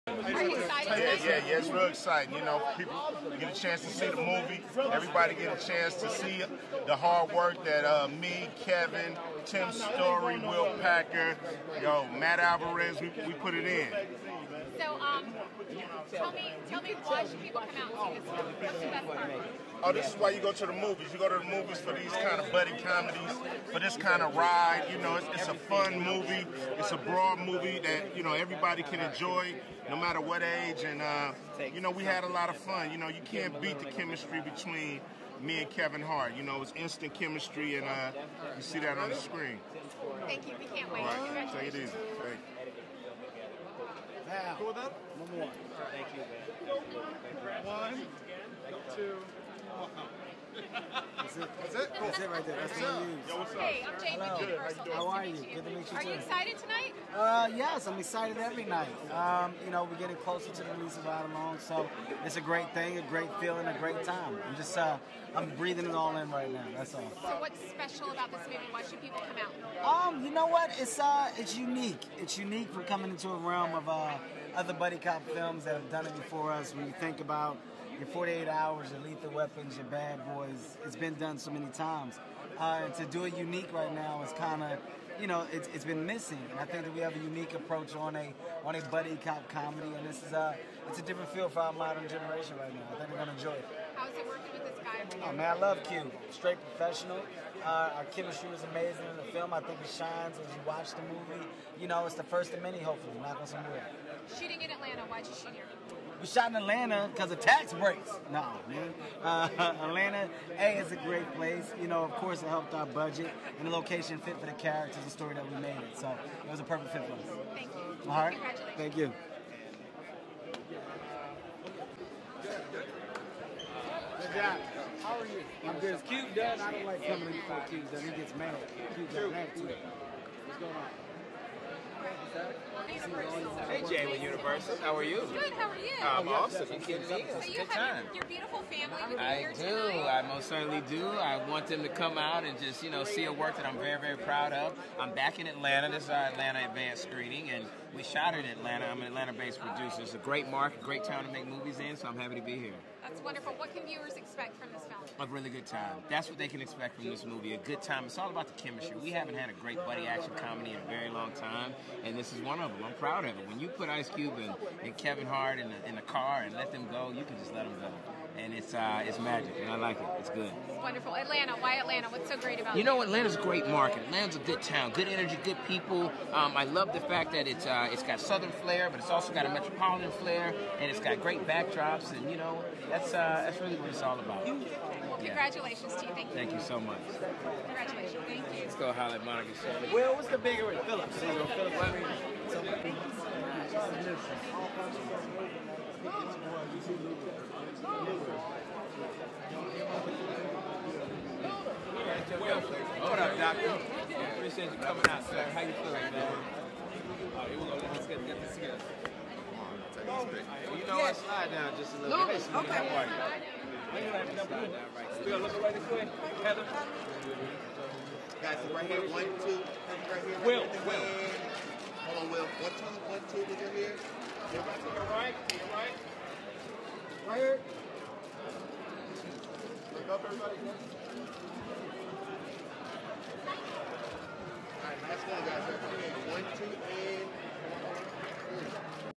Ride Along ATL Red Carpet
Check out the Red Carpet held in Atlanta for "Ride Along," starring Ice Cube and Kevin Hart.

Movie Info
Kevin Hart and Ice Cube lead the lineup in Ride Along, the new film from the director and the producer of the blockbuster comedy Think Like a Man. When a fast-talking guy joins his girlfriend's brother-a hot-tempered cop-to patrol the streets of Atlanta, he gets entangled in the officer's latest case. Now, in order to prove that he deserves his future bride, he must survive the most insane 24 hours of his life.